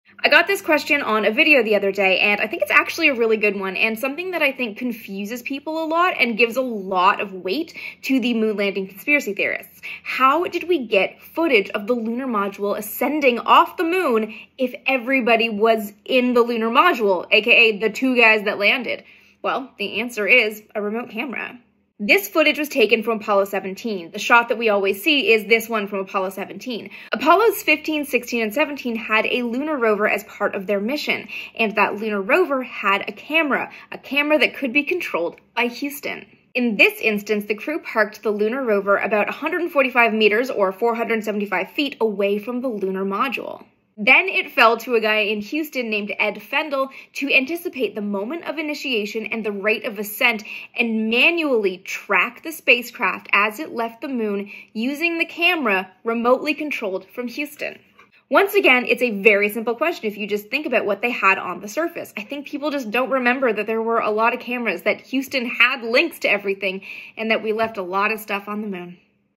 Who filmed the Lunar Module leaving the Moon?
Apollo missions left a lot of stuff on the Moon, including a lunar rover and a camera that could be remote controlled!